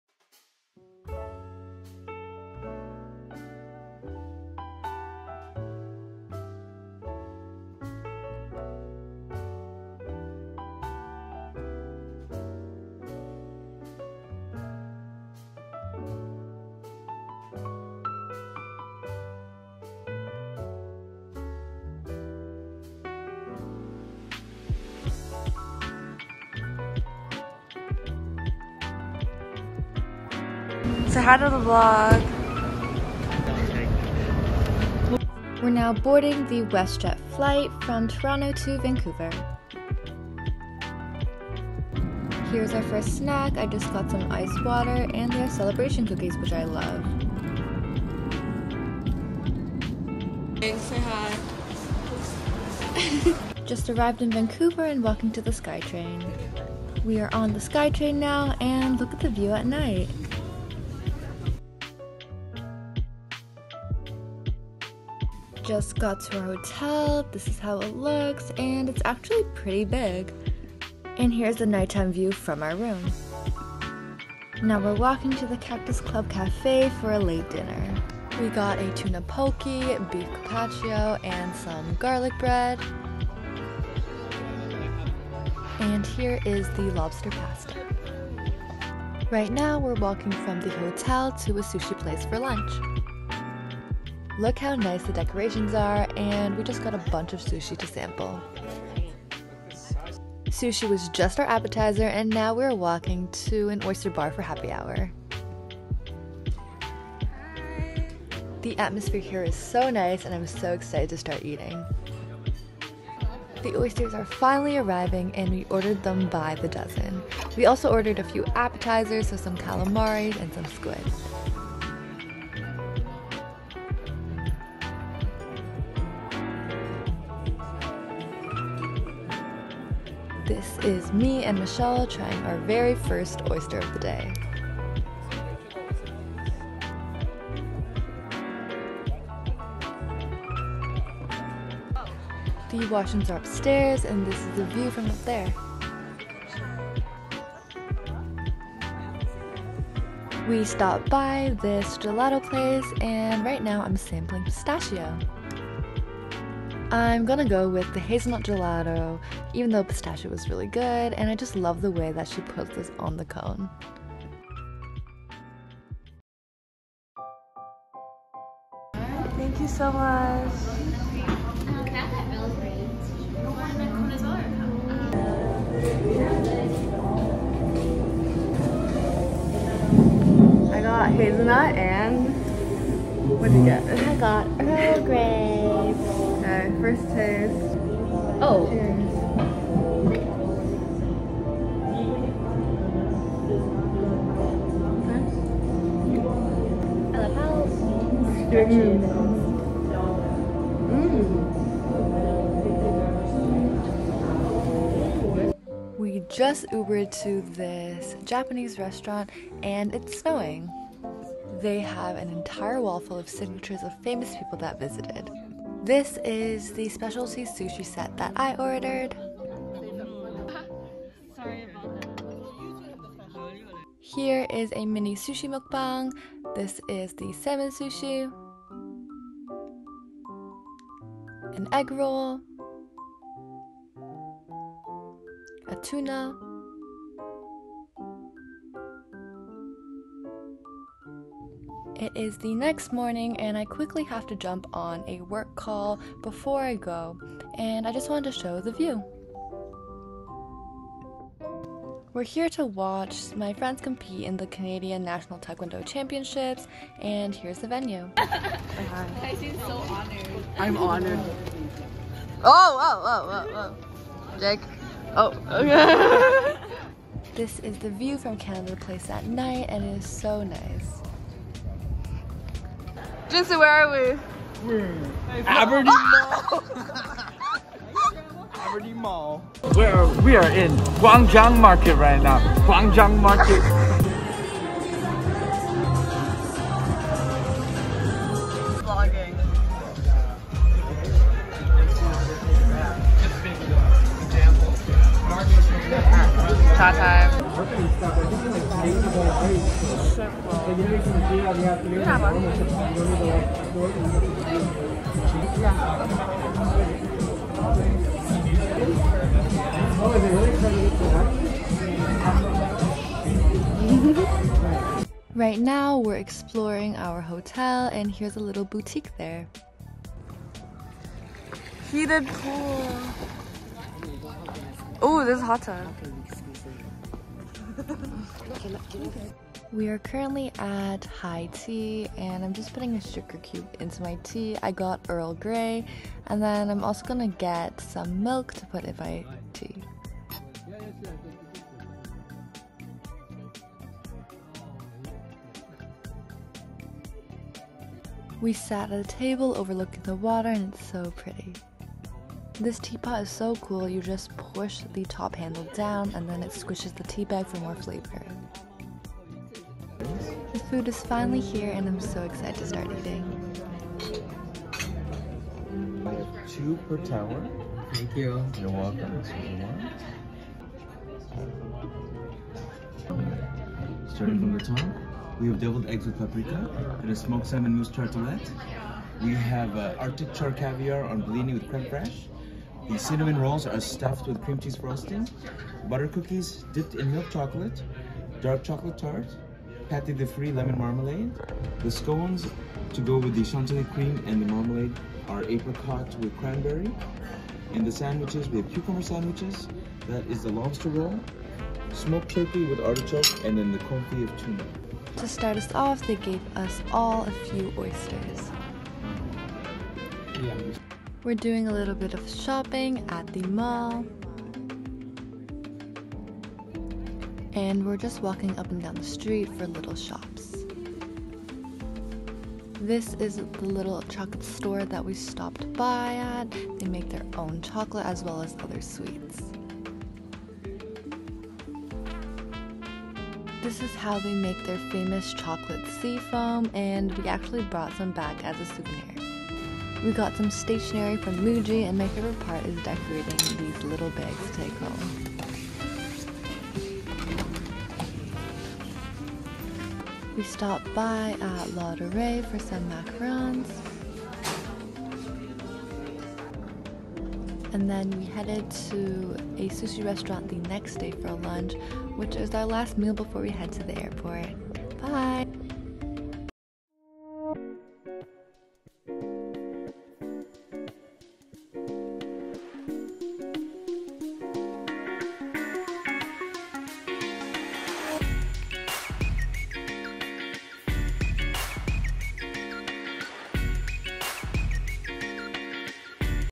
WHAT TO DO IN VANCOUVER (VLOG!) 2023 | travel, student, cafe, seafood, oyster, food, downtown
hellooo! we are going to vancouver!! we ate so many delicious foods, especially seafood and oysters. we did lots of shopping and had so much fun. it would mean the world if you could SUBSCRIBE!

love,
sabrina

editing and music:
iphone 13
ipad air 4
apps ( vllo, canva, procreate )